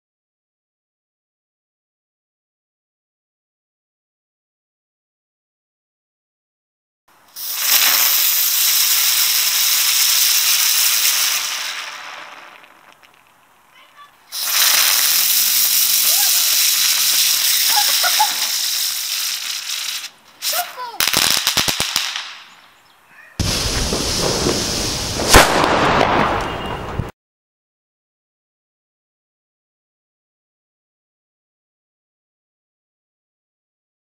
Welke stukjes vuurwerk mag de winkelier aan je verkopen als je 12 jaar oud bent
Peter van der Sluijs heeft dit lesfilmpje gemaakt voor bijv. scholen.
Welke stukjes vuurwerk is Cat 1 vuurwerk.
Cat 1 vuurwerk mag je het hele jaar kopen en afsteken mits je 12 oud bent.